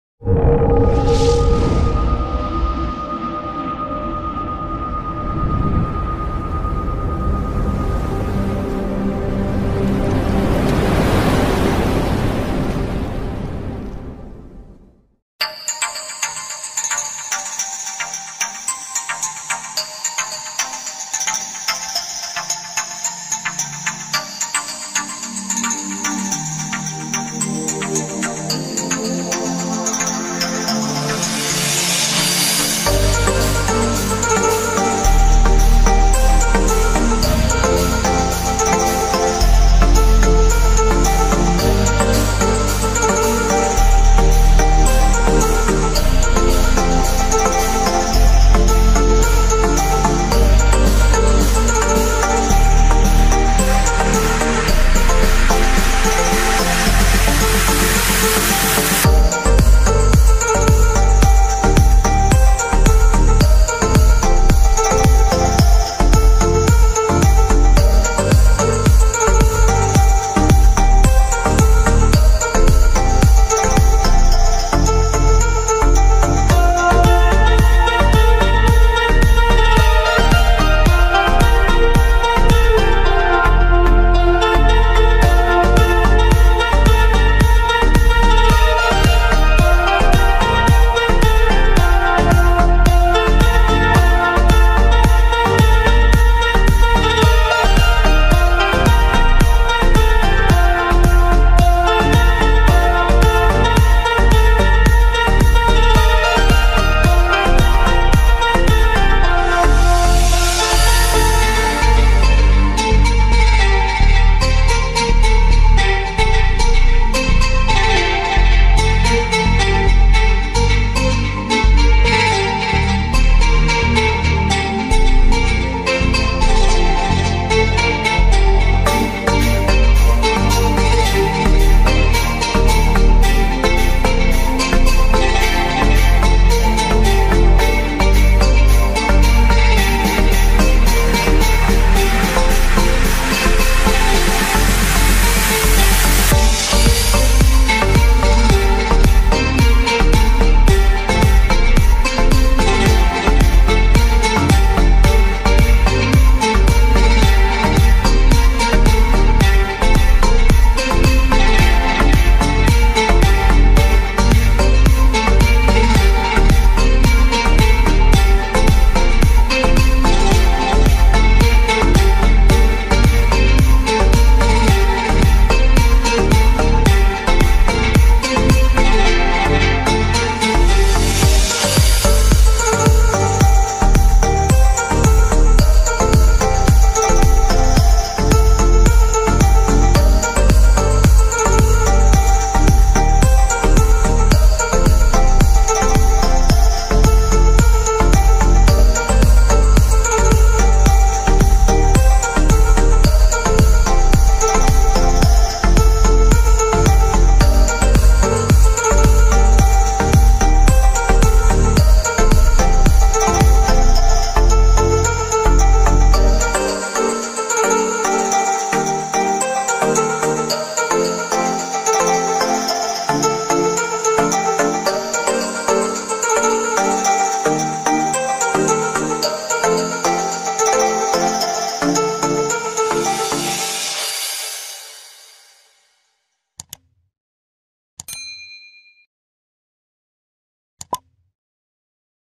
جولة في الطريق السريع بين السعيدية وأحفير ،والدخول الى وجدةTour de l'autoroute entre Saïdia et Hafir
جولة في الطريق السريع بين السعيدية وأحفير ،والدخول الى وجدة
Un tour de l'autoroute entre Saïdia et Hafir, et entrée à Oujda